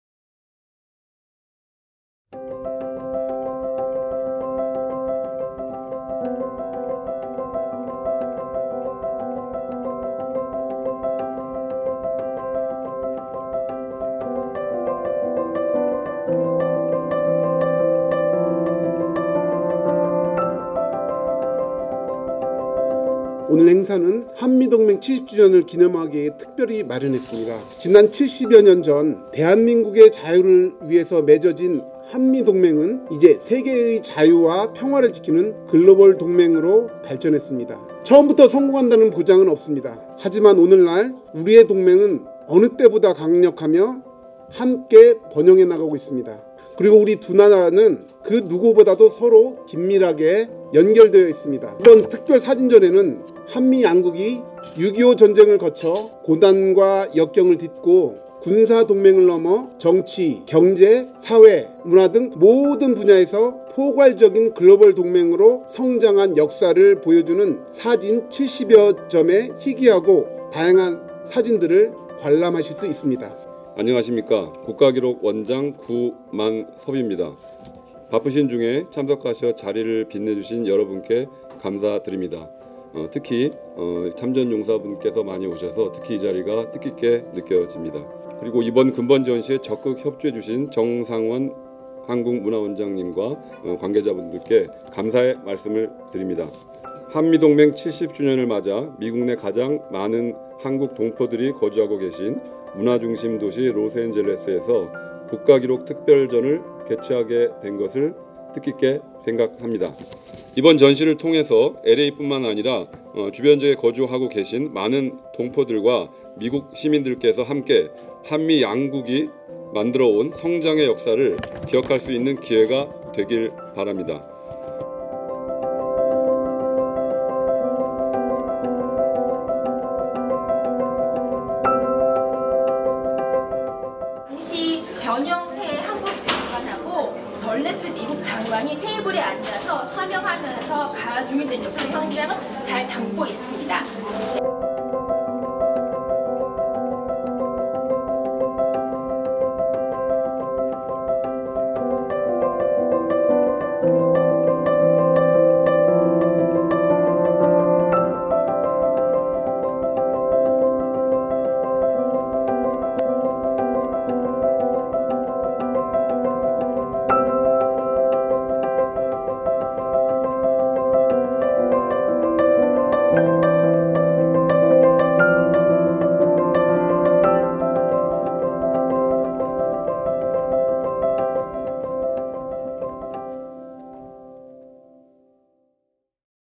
A Special Photography Exhibition: 70 Years of ROK-U.S. Alliance in Photos, “We Go Together!”
- Title: A Special Photography Exhibition: 70 Years of ROK-U.S. Alliance in Photos, “We Go Together!”
- Exhibition Presenters: Korean Cultural Center, Los Angeles, Korean Ministry of the Interior and Safety's National Archives of Korea

KCCLA provides online content 'Exhibition Highlights' through our website & YouTube channel for the KCCLA audience.

The Korean Cultural Center Los Angeles (KCCLA), in partnership with The Korean Ministry of the Interior and Safety's National Archives of Korea, proudly presents a special photography exhibition: 70 Years of ROK-U.S. Alliance in Photos, “We Go Together!” at the Korean Cultural Center Art Gallery (located at 5505 Wilshire Blvd.

The main theme of the exhibition, "We Go Together" symbolizes the alliance between ROK-U.S., since ROK General Paik Sun-yup (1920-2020) met his ally, U.S. General Douglas MacArthur, in February, 1951 and said those exact words. 2023 marks the 70th anniversary of the October 1st, 1953 signing of the Mutual Defense Treaty between the United States and the Republic of Korea.

The National Archives of Korea created this special collection of historical photos and media to be exhibited in the United States to commemorate this important milestone. More than 70 photos show the history of Korea and the United States overcoming hardships and adversity through the difficult years of the 1950-53 Korean War, emerging later as an ironclad alliance. They also illustrate how this unique alliance has endured through time, extending beyond the military to include shared civic values, economic interests, and cultural ties.